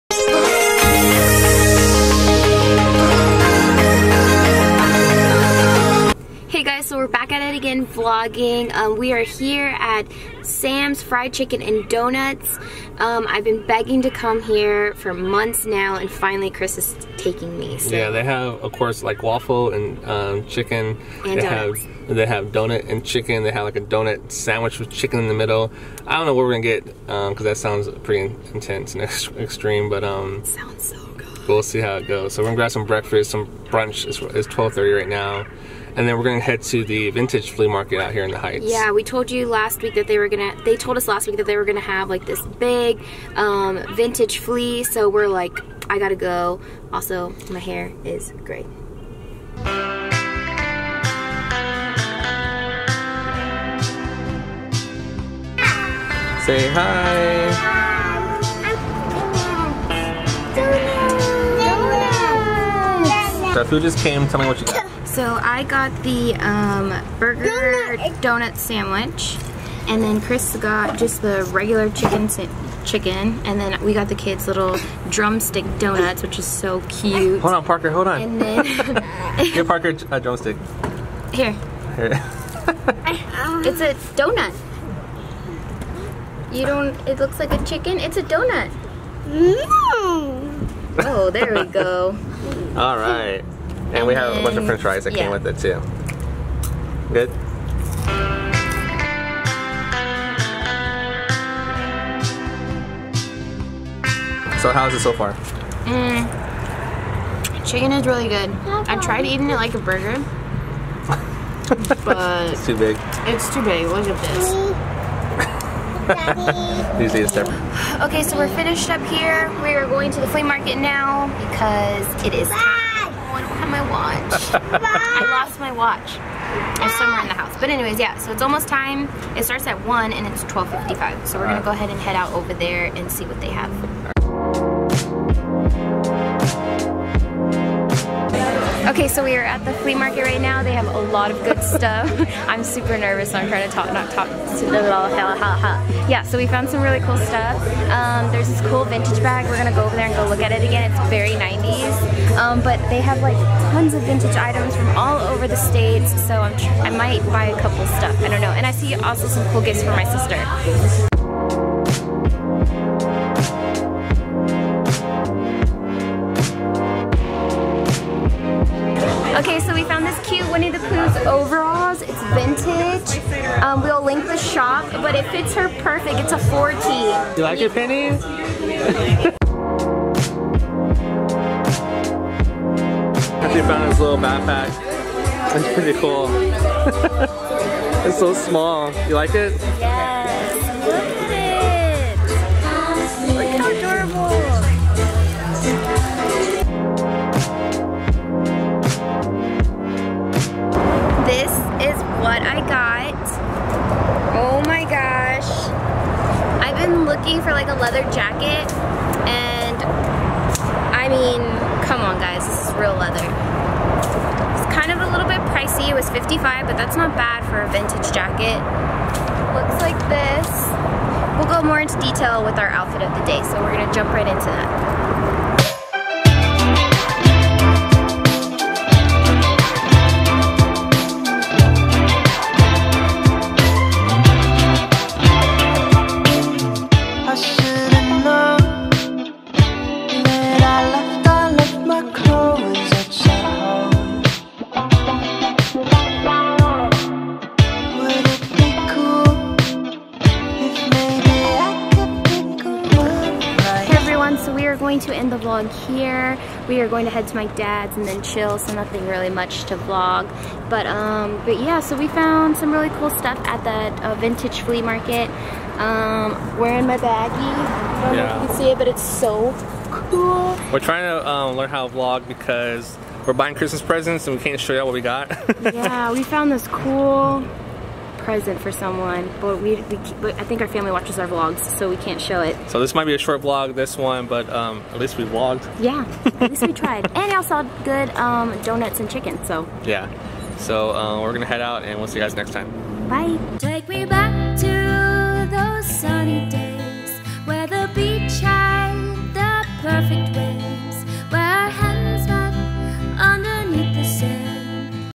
Donut Burgers & Vintage Flea Market
Hey everyone, Vlog 29 is here! Sorry it's so short, lost some footage. Hope you enjoy!

Jacket From : Intertwined Vintage
90s Backpack: Yoko Vintage
Pennys Pooh Overalls : Sundried Vintage